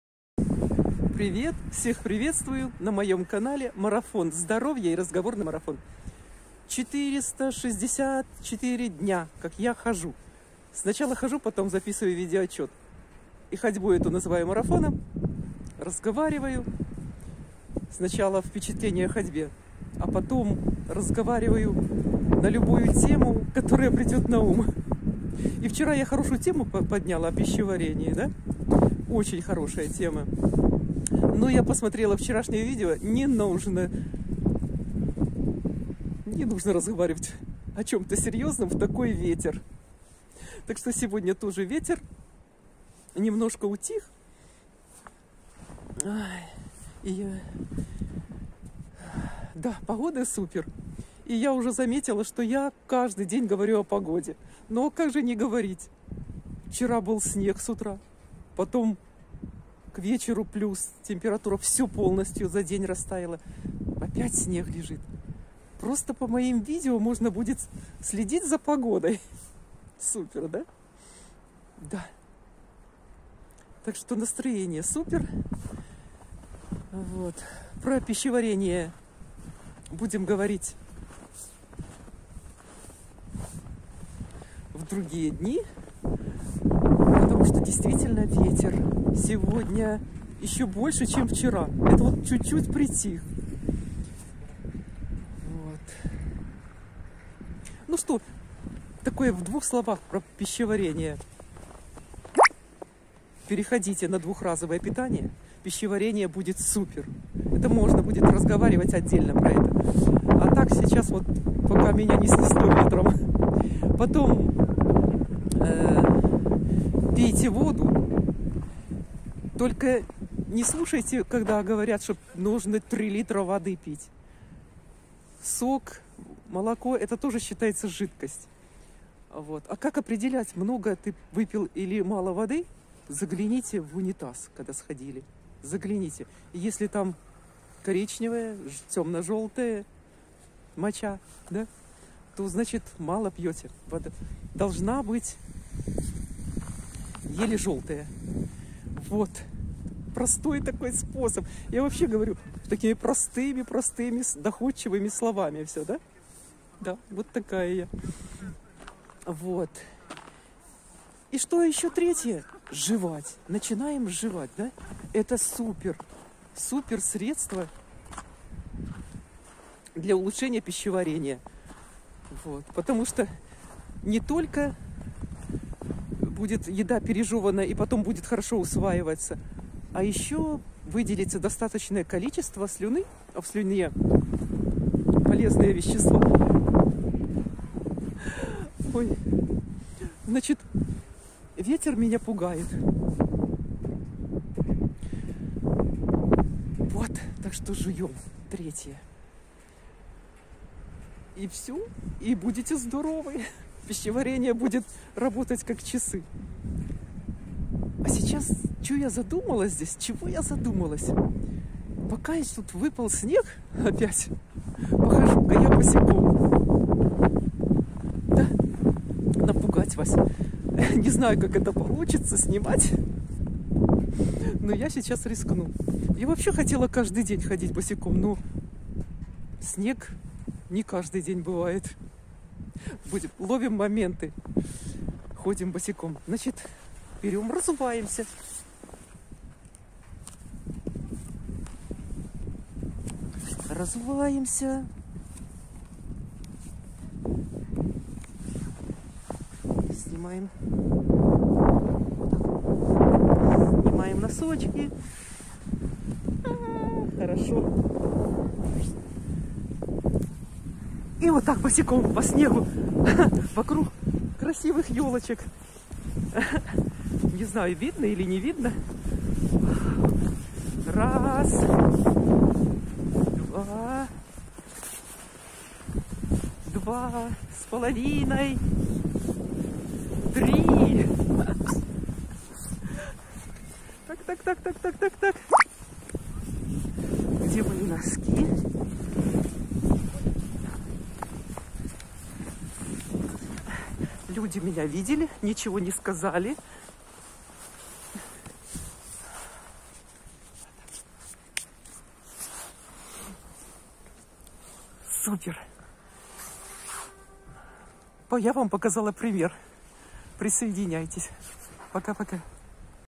Для начала улучшения пищеварения... "Марафон здоровья и разговорный марафон' день 464
Всех приветствую на моем канале Марафон здоровья и разговорный марафон. День 464. Хожу каждый день и свою ходьбу называю марафоном.  Сегодня опять ветер. Скажу кратко о вчерашней теме Пищеварение. Что нужно для хорошего пищеварения: 1) Для начала пейте достаточно жидкости. 2) Попробуйте перейти на двух разовое питание. 3) Начните лучше пережёвывать пищу.